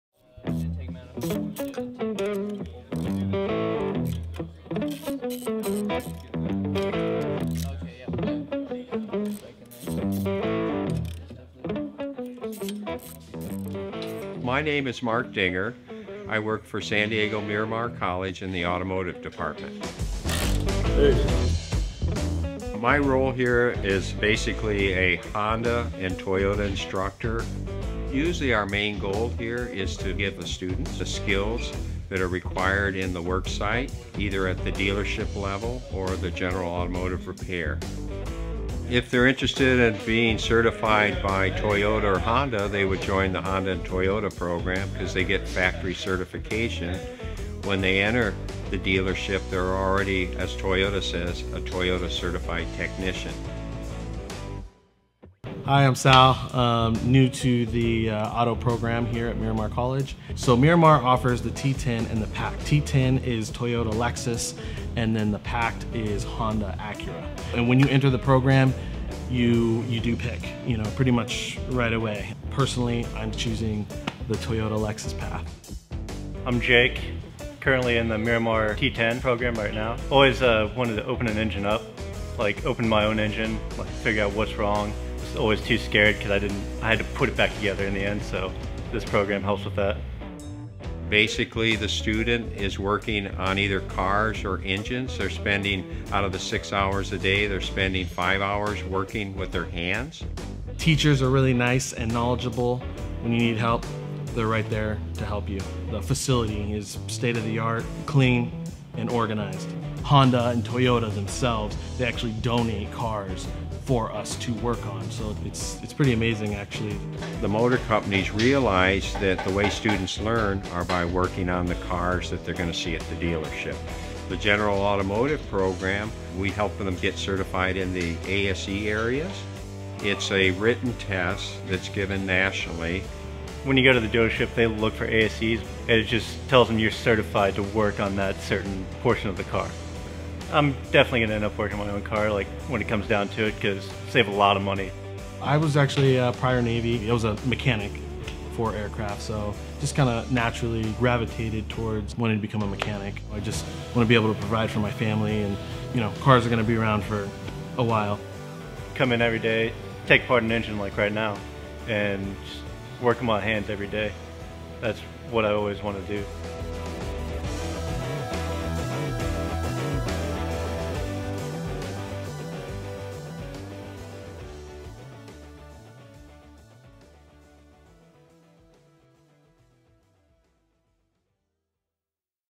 San Diego Miramar College - Automotive Technology Program
San Diego Miramar College offers a state-of-the art Automotive Technology Program, dedicated to advancing technical and workforce education, as well as training in advanced transportation technologies.

Miramar has two primary programs under Automotive Technology: General Automotive and Honda PACT/Toyota T-Ten.

Visit sdmiramar.edu to learn more about this program and others!